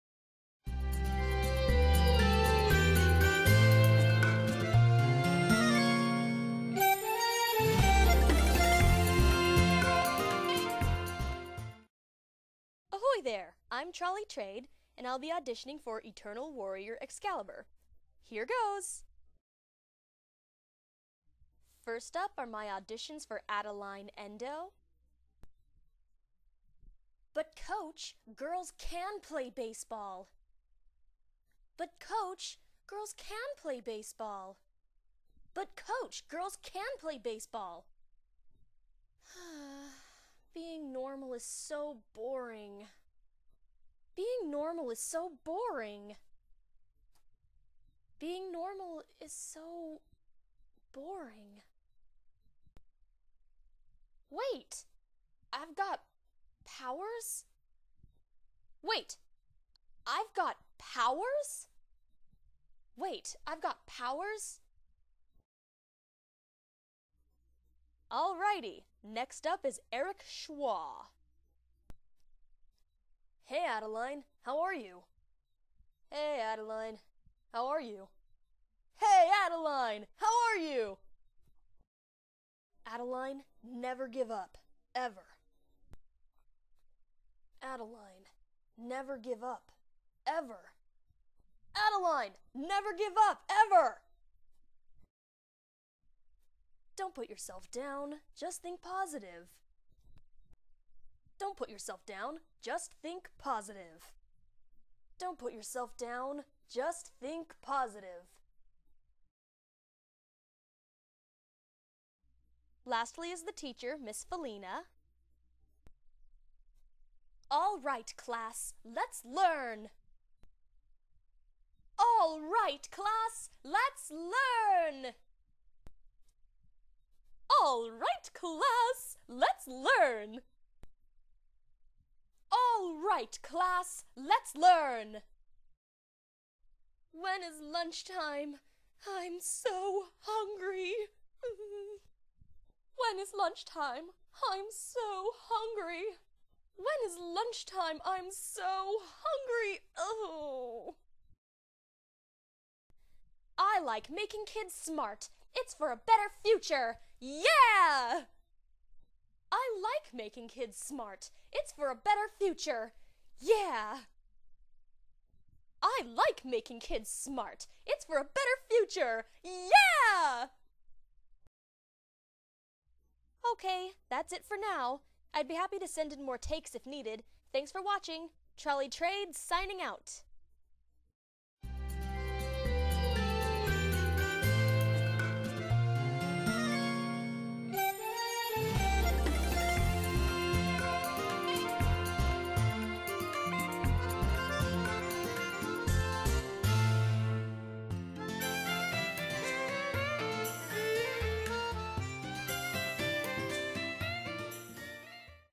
Trolli Trade - Eternal Warrior Excalibur Audition
Hey there! I'm at it again! ^_^

Here are my auditions for Taralen's "Eternal Warrior Excalibur". I noticed that Adeline Endoh, Eric Schwa, and Ms. Falina were still open roles, so I decided to try all three of them. ^_^ Wish me luck!

I've been to the website, so I know that the actual episodes for the show are... postponed... for workload reasons, but I'll try out anyway. ^_^ Taralen mentioned she may still do shorter animated projects, and plus, you never know when you'll need a full voice cast for a show. Something cool might come up!

Anyway, this sounded like fun, so I put this audition video together today. I'm happy to announce that it is mic-pop free with good sound quality. xD

~Trolli!

P.S...

If anyone's wondering why I keep auditioning for male roles too, it's 'cause I always play guy characters when I do voices with my friends. ^_^;; I'm just sort of used to voicing male characters, so I'll usually audition for both genders. ^_^